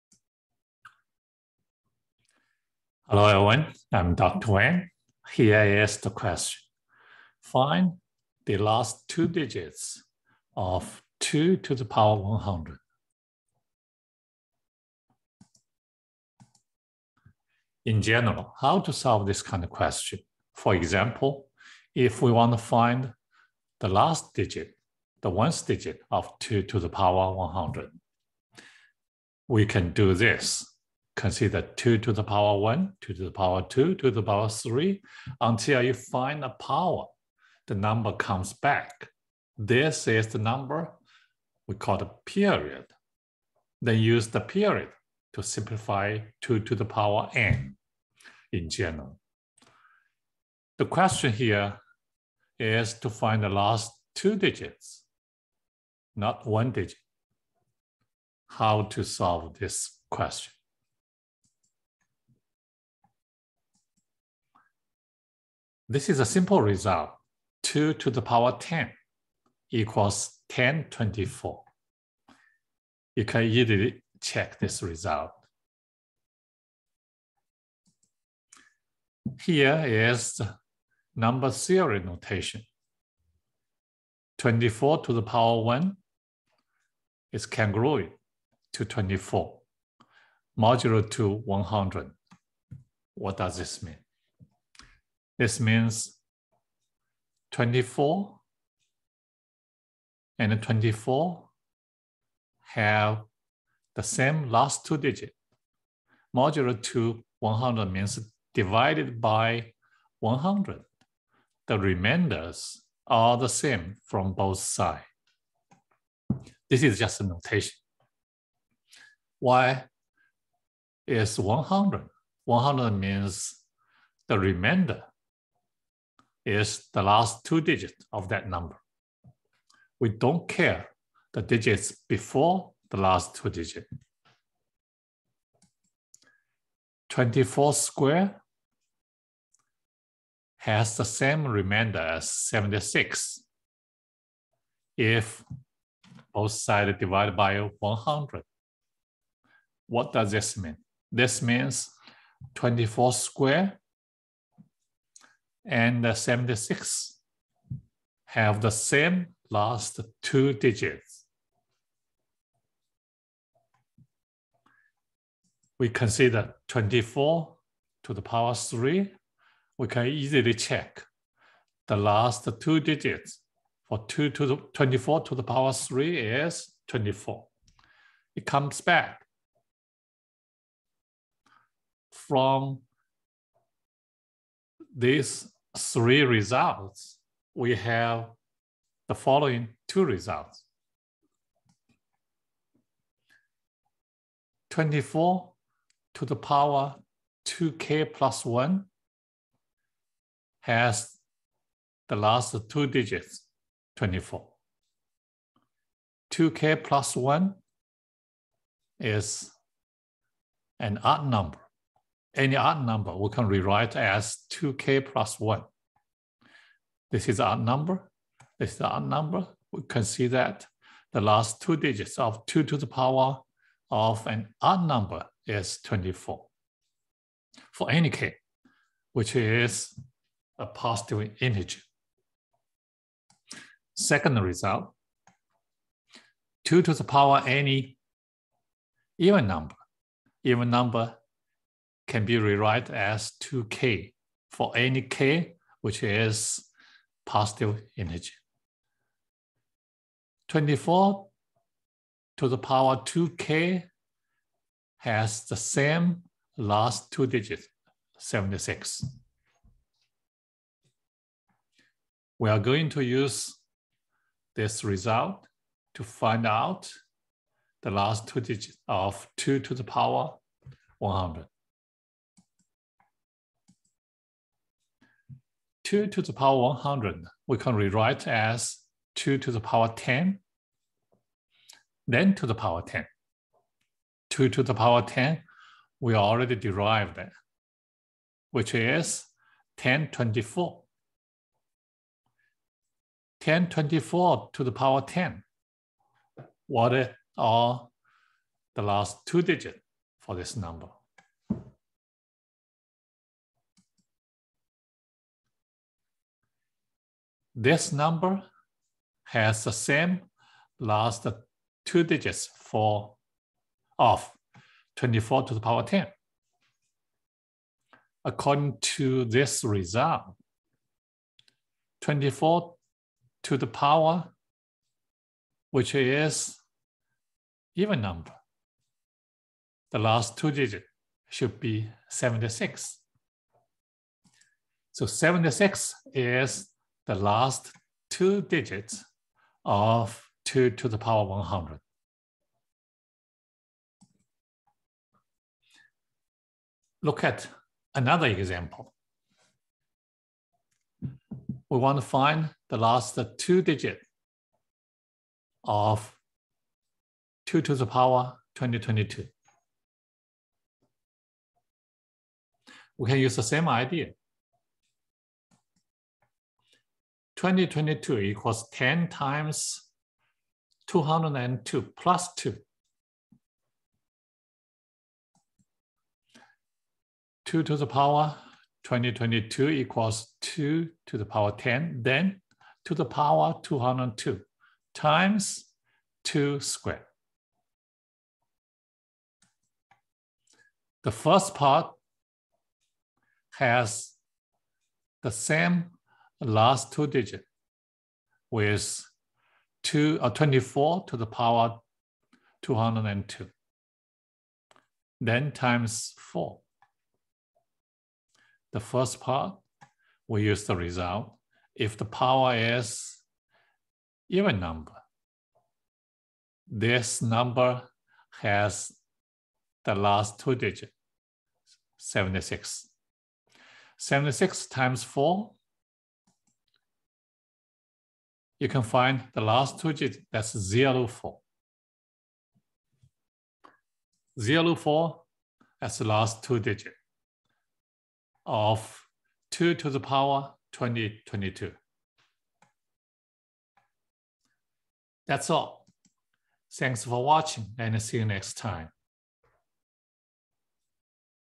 Find the Last Two Digits of 𝟐^𝟏𝟎𝟎
We used the following results to find out the final answer:
2^10 = 1024, the last two digits of 24^(odd) are 24, and the last two digits of 24^(even) are 76.
We also discussed how to find the last two digits of 2^2022.